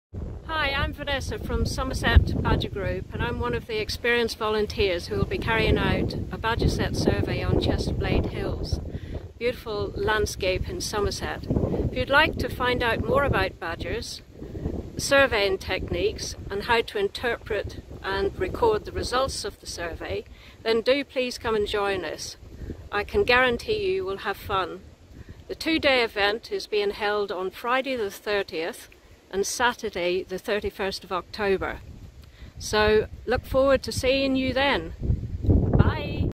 Badger Survey Day at Chesterblade Hills
Listen to Vanessa from Somerset Badger Group telling us what to expect at the Badger Survey Day at Chesterblade Hills.